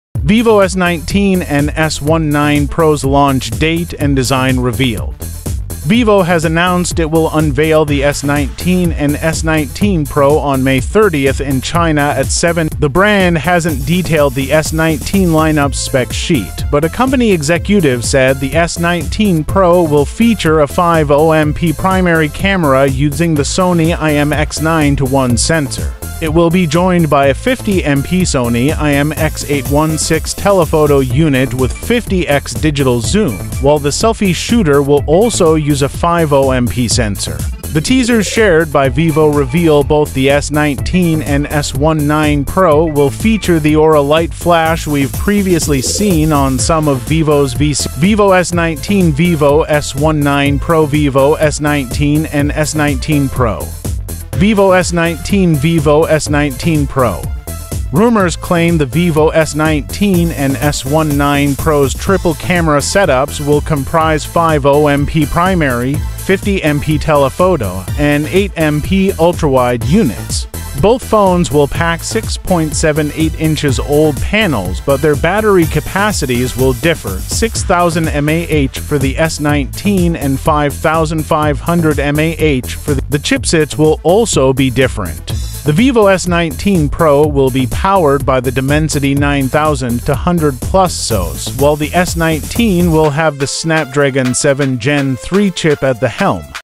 vivo S19 & S19 Pro Launch: 50MP Camera, May 30 Release!
Get ready for the exciting launch of the vivo S19 and S19 Pro, set to be unveiled on May 30 in China at 7 PM local time! This event promises to reveal the stunning design and advanced features of the S19 lineup. While the full specs sheet remains a mystery, vivo has confirmed that the S19 Pro will boast a 50MP primary camera with the Sony IMX921 sensor, a 50MP telephoto lens with 50x digital zoom, and a 50MP selfie shooter. The S19 and S19 Pro will also feature the Aura Light flash, a signature from vivo's V-series.

Rumors suggest both models will sport 6.78" OLED panels and triple camera setups, including 50MP primary, 50MP telephoto, and 8MP ultrawide units. The S19 will come with a 6,000 mAh battery and Snapdragon 7 Gen 3 chip, while the S19 Pro will have a 5,500 mAh battery and Dimensity 9200+ SoC.